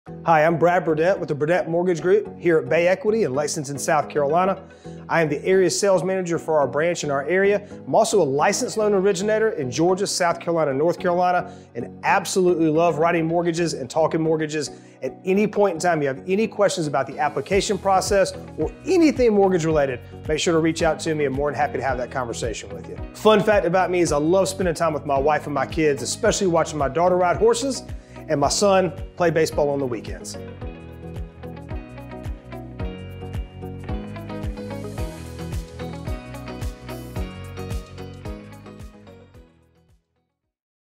Meet Brad Burdette
Meet Brad Burdette- Loan Officer and Area Sales Manager
NMLS#194126
Serving South Carolina, North Carolina and Georgia.